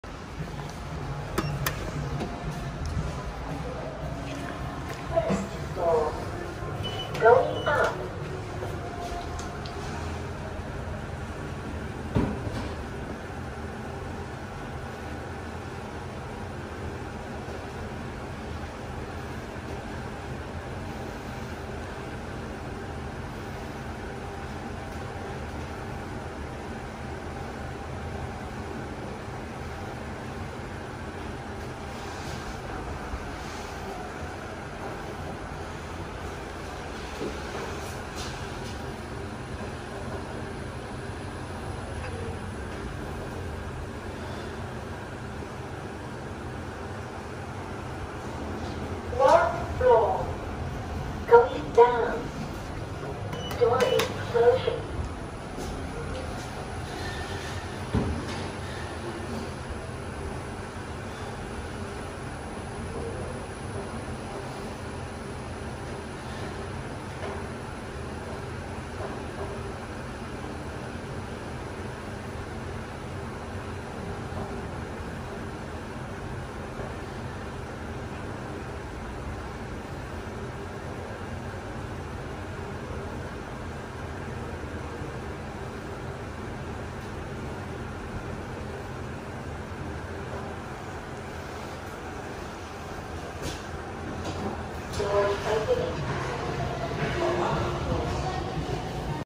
OTIS GEN2 REGEN MRL @ EUNOIA JUNIOR COLLEGE [LOBBIES A2 & A3 STUDENT LIFTS]
Lobbies A2 and A3 go the full 12 floors of the building. These have two lift service patterns, with the staff lift pattern having all floors available (though behind a keycard door that students cannot pass through), and the student lift pattern (this video) only allowed to serve certain floors, even though all the lifts in these two lobbies have landings at every floor, possibly to allow configuration of service patterns (which is what happened to the Otis lifts at Broadway Plaza when both lifts are allowed to serve all floors instead of each lift serving selected landings).

Between both lobbies, the student lift service pattern for lobby A2 differ from that in lobby A3; the lifts meant for students in lobby A2 stops at floors 1, 5, 9 and 12, while those at A3 stops at floors 2, 4, 7 and 10.